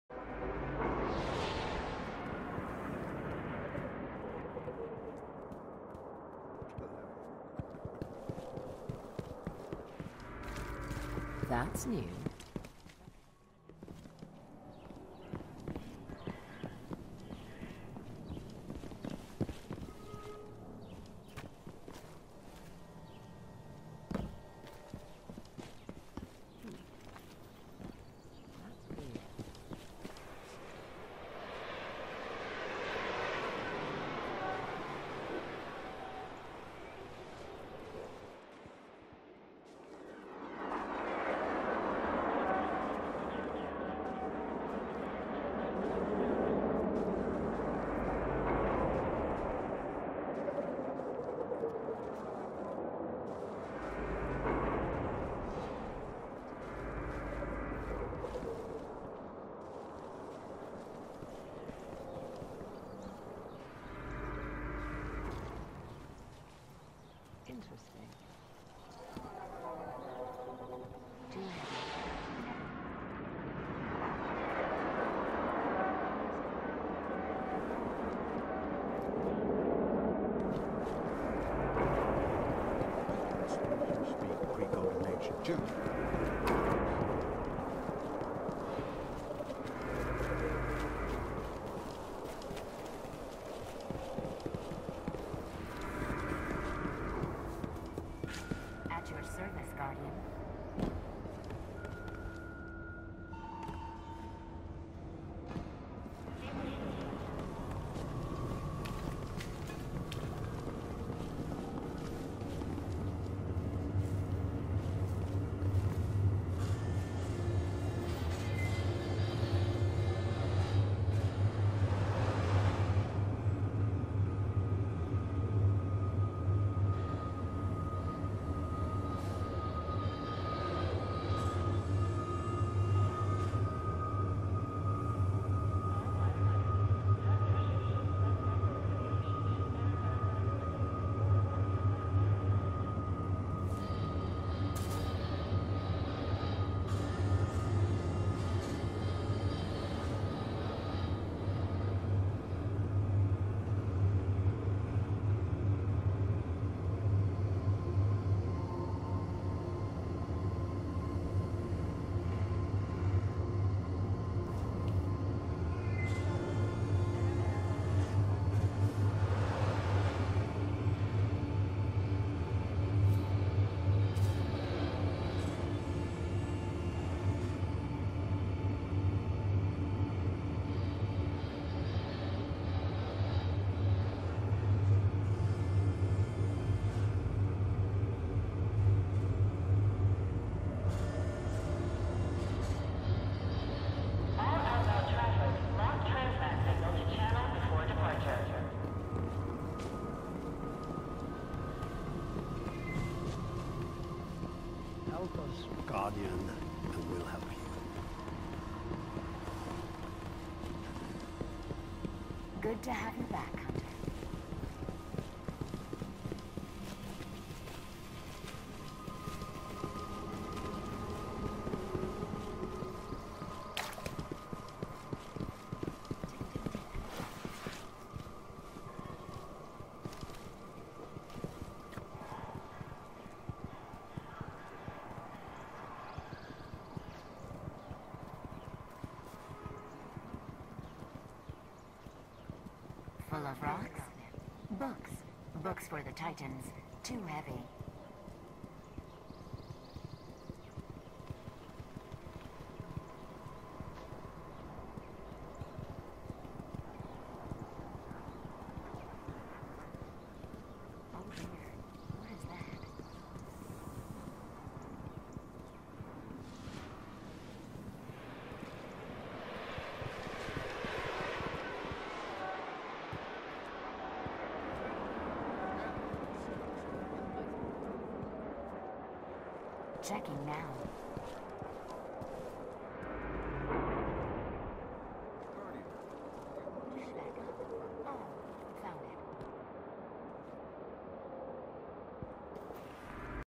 Prequel to the Dance Party (Alpha's goodbye)
Me and Penguin going on an adventure before starting our dance circle